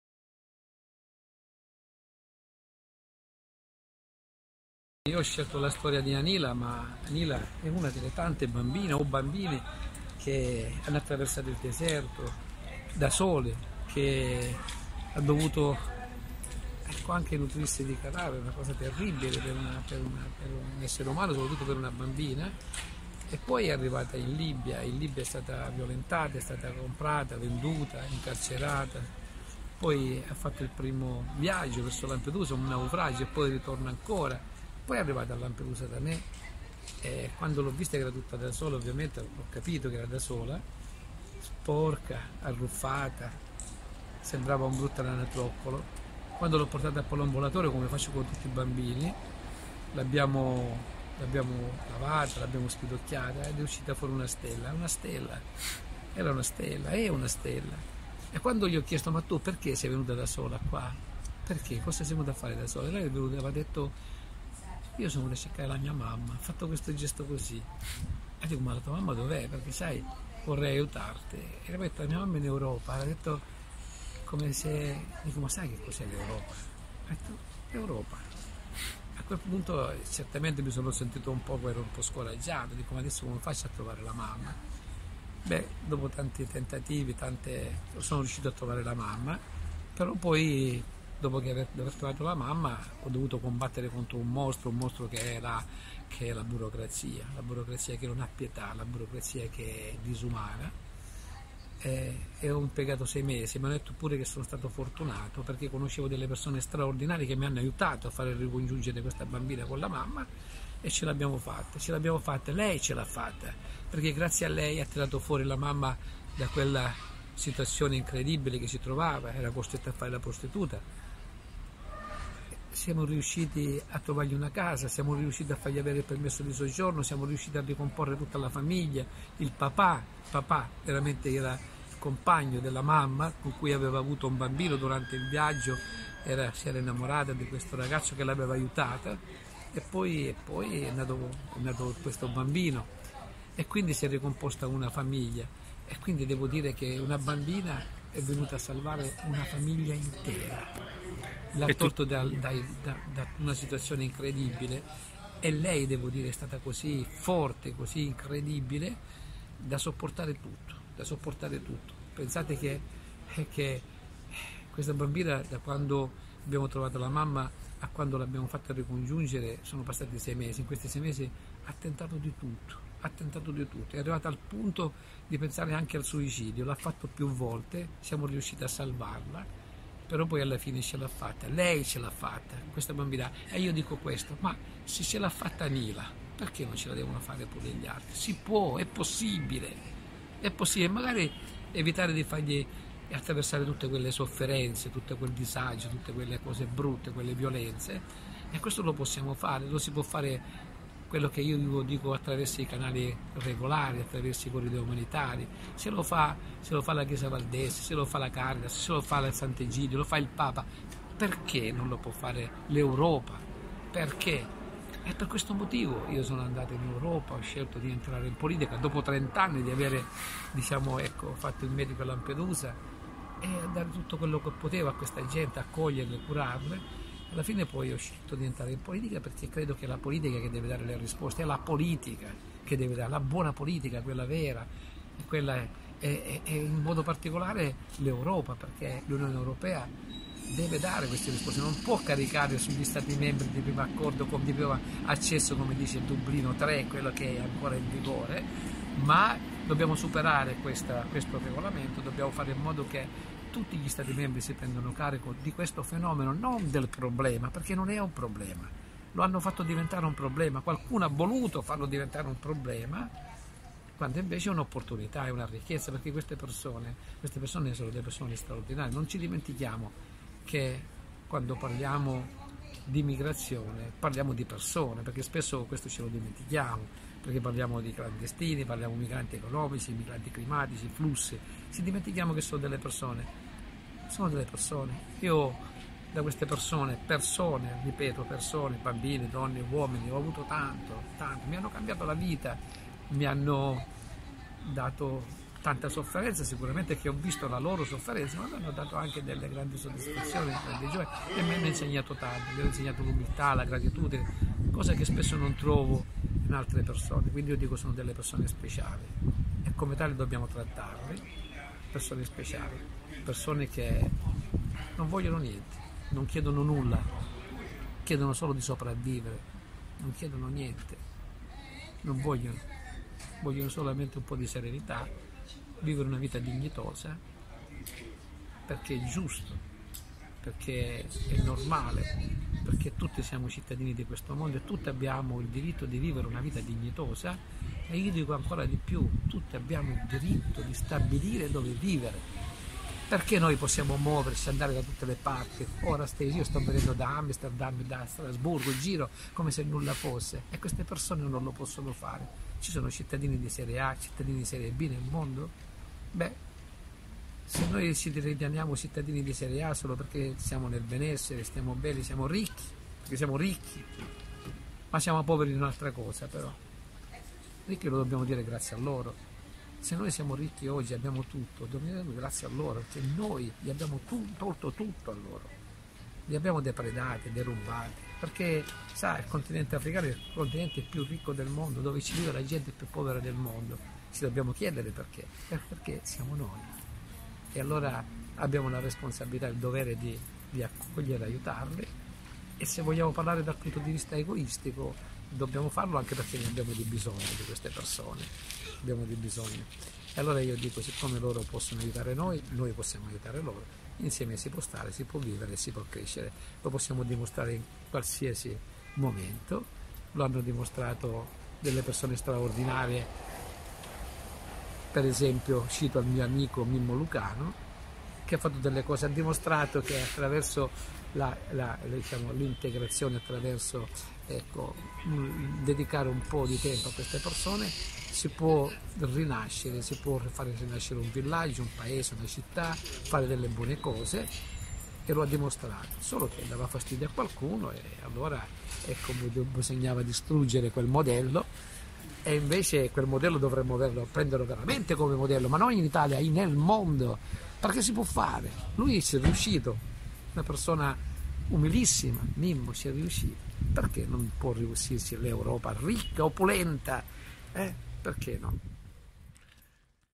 Intervista a Pietro Bartolo
Pietro Bartolo, il "medico di Lampedusa" oggi europarlametare, racconta la sua esperienza